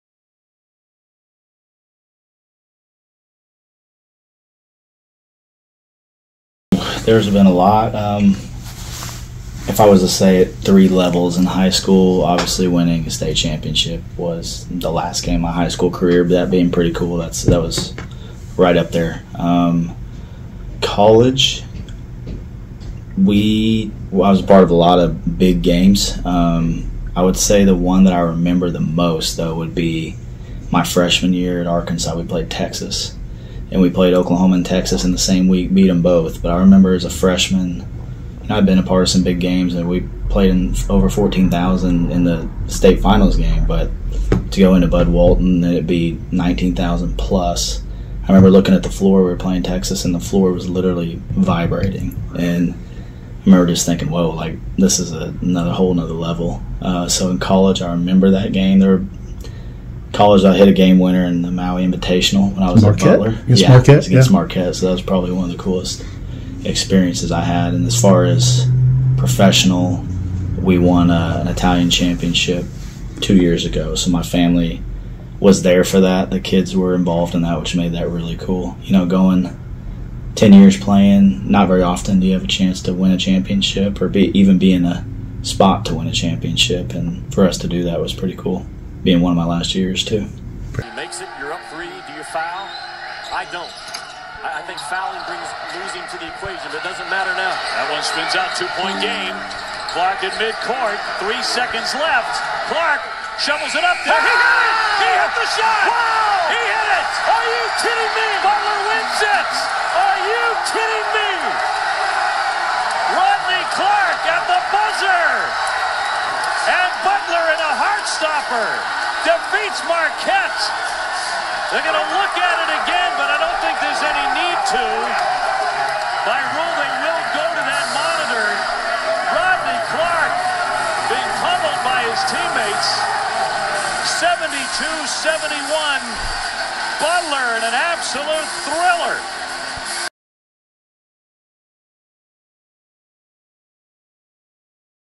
HEY ROTNEI! "What are the greatest games you have ever been a part of?"
We ask Rotnei Clarke, the OKLAHOMA ALL-TIME BOYS BASKETBALL career scorer a few questions...